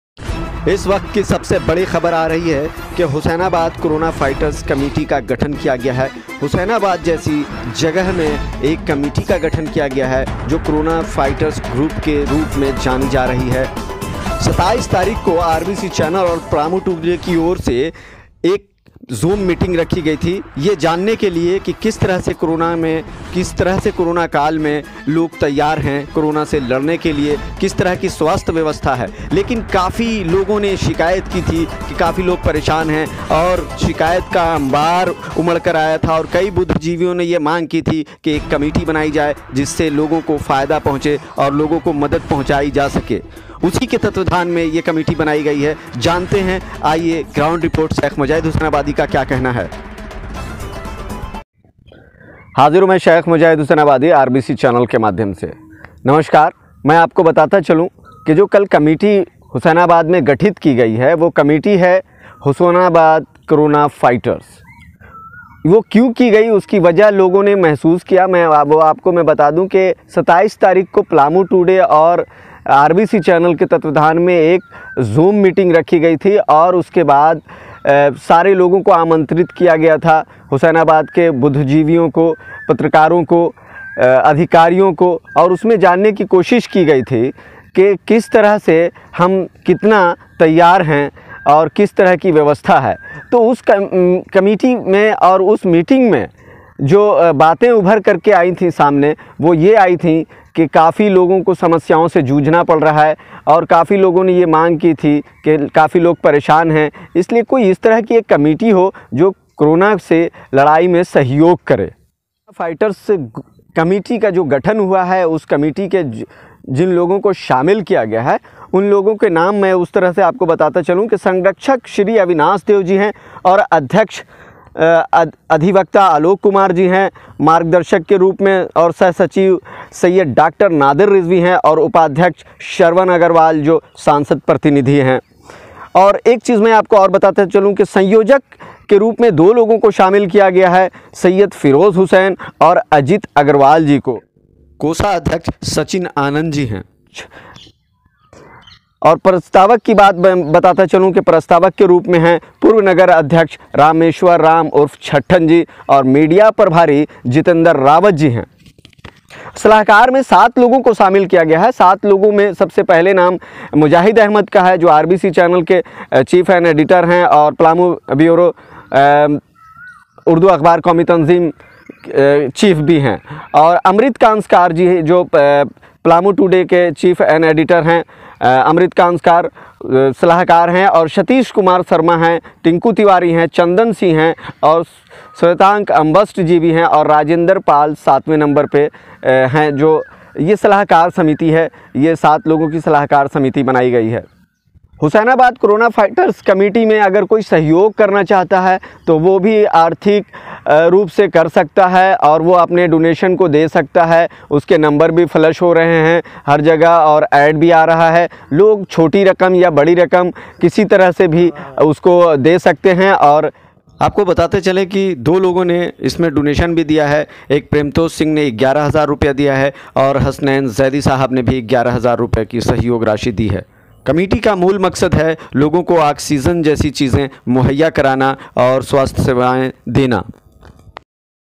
लोगों को स्वस्थ सुविधा देने हेतु बनी कमिटी ,,हुसैनाबाद कोरोना फ़ाइटर,,
आक्सीजन की कमी से मरते लोगों को बचाने के लिए ,कोरोना से लड़ने के लिए ,लोगों की मदद करने के लिये बनी,  हुसैनाबाद कोरोना फ़ाइटर,कमिटी